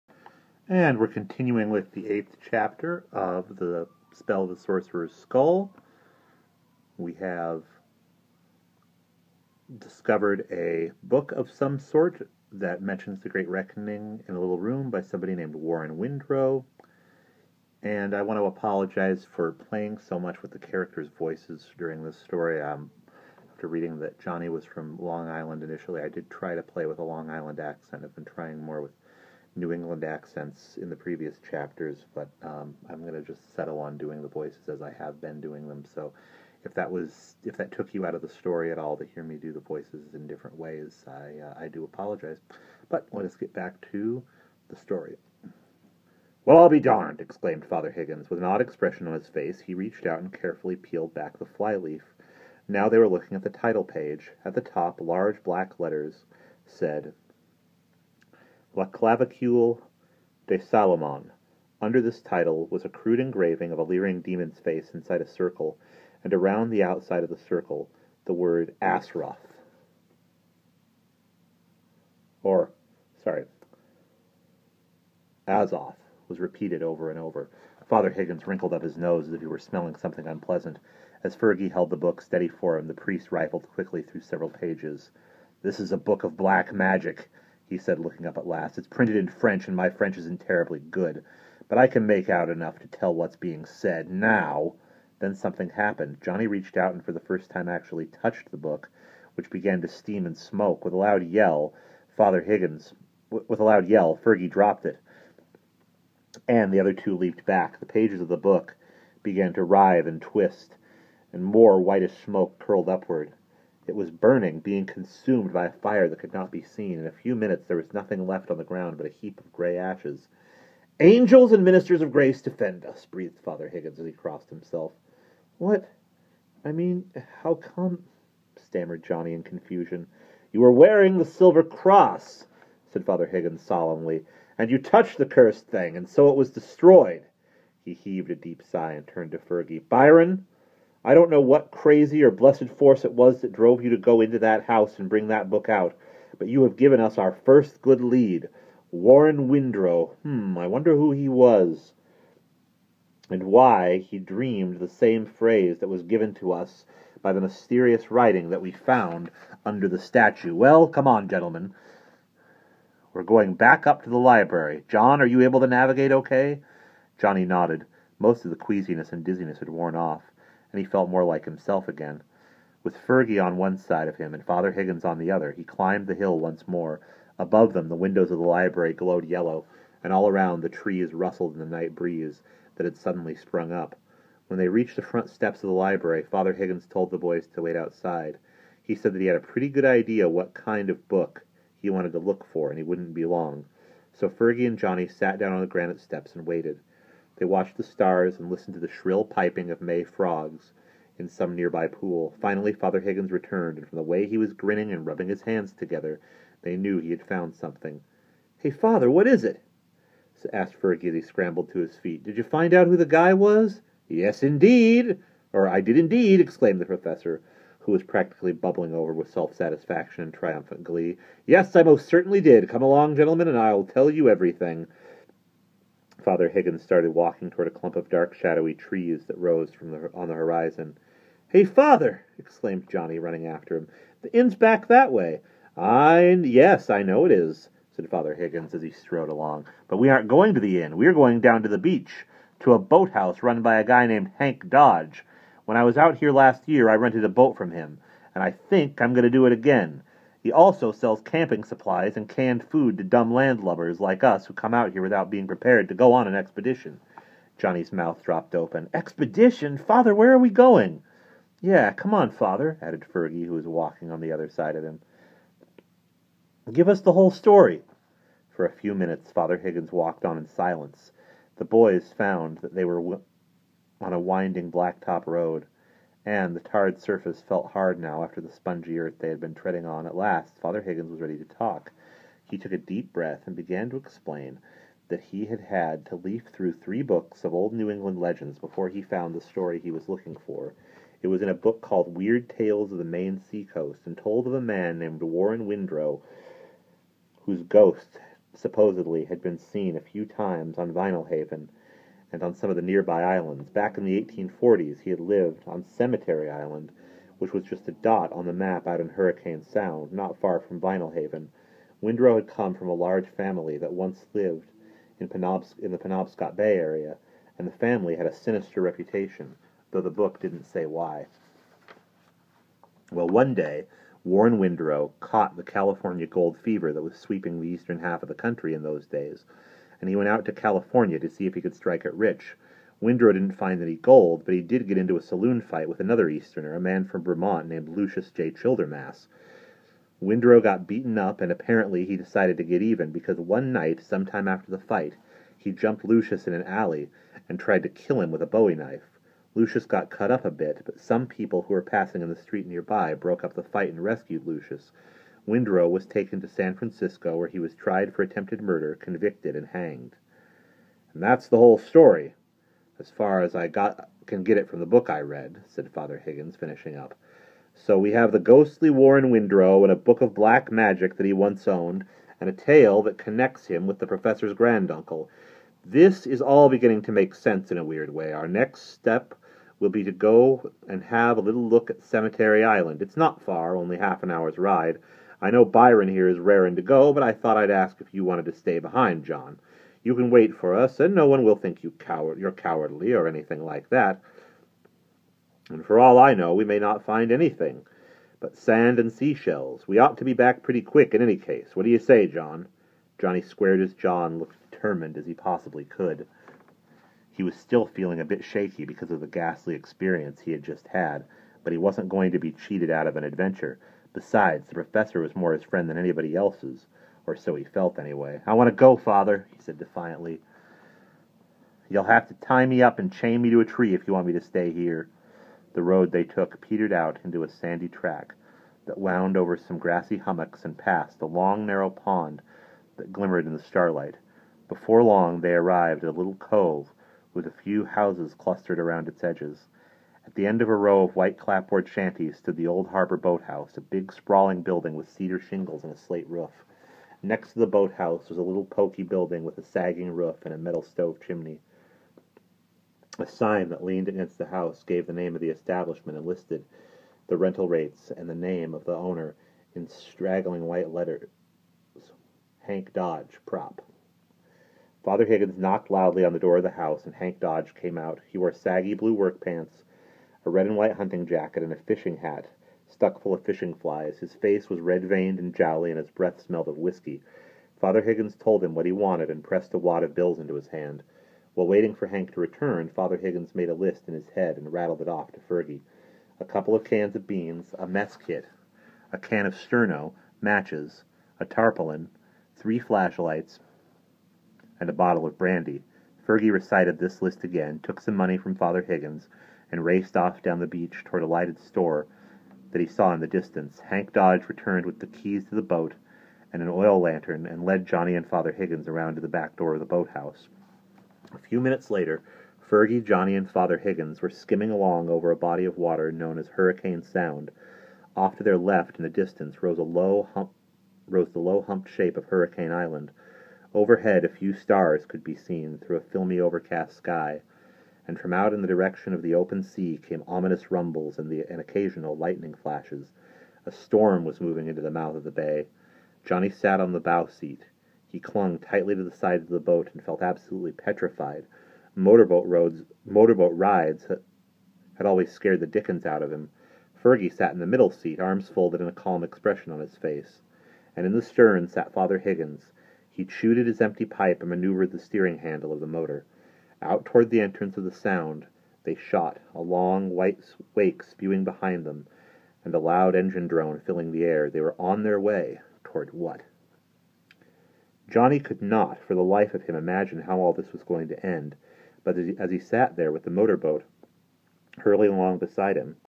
John Bellairs 'The Spell of the Sorcerer's Skull' Chapter Eight continues
I continue to read you the eighth chapter of 'The Spell of the Sorcerer's Skull'